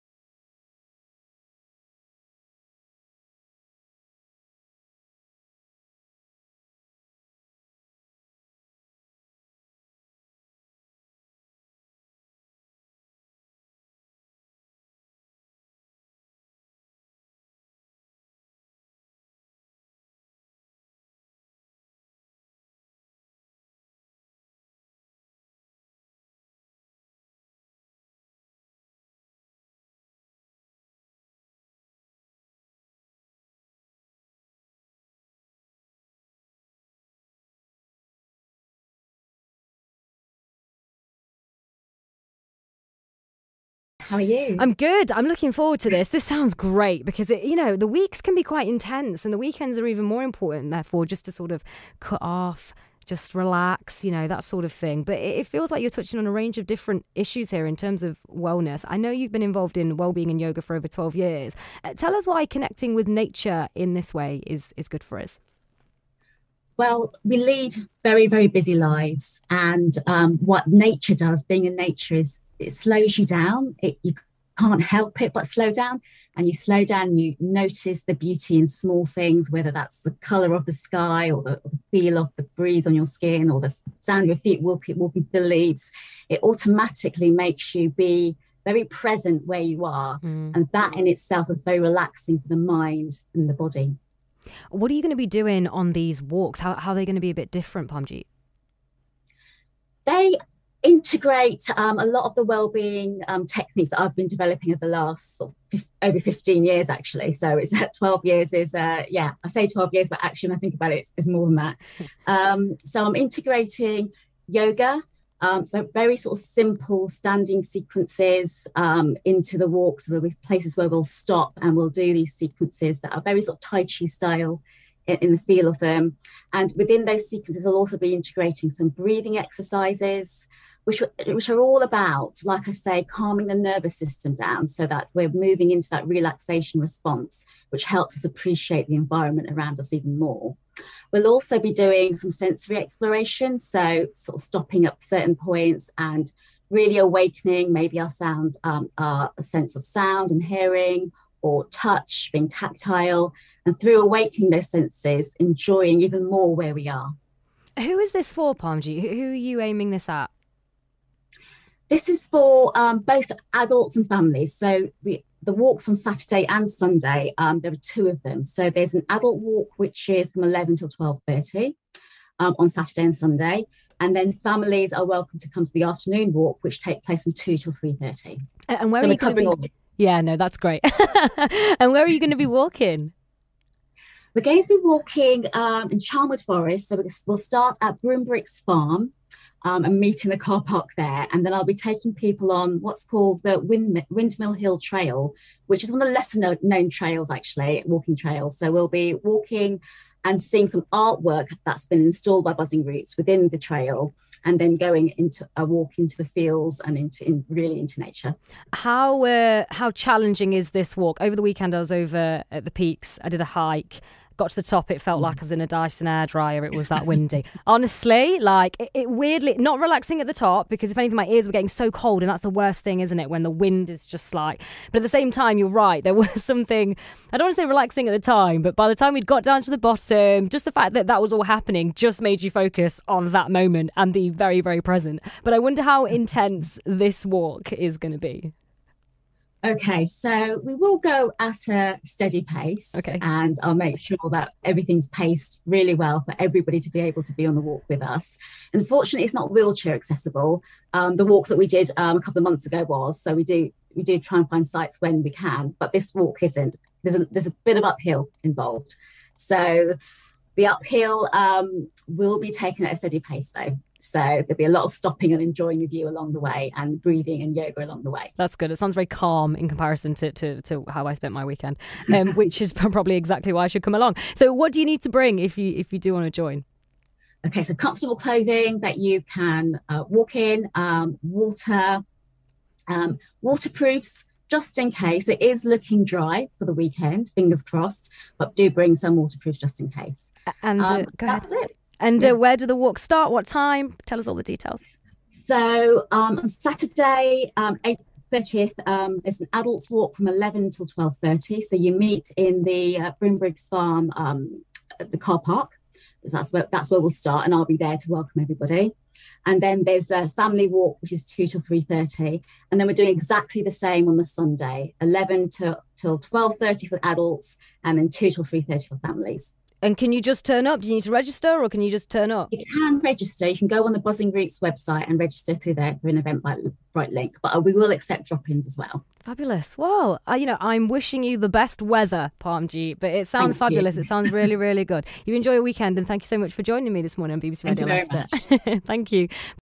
Buzzing Roots mindfulness walks at Beacon Hill | BBC Radio Leicester
Summaya Mughal of BBC Radio Leicester talks to Parmjit Sagoo about Buzzing Roots mindfulness walks at Beacon Hill Saturday 30 April & Sunday 1 May 2022.

Supported by The National Forest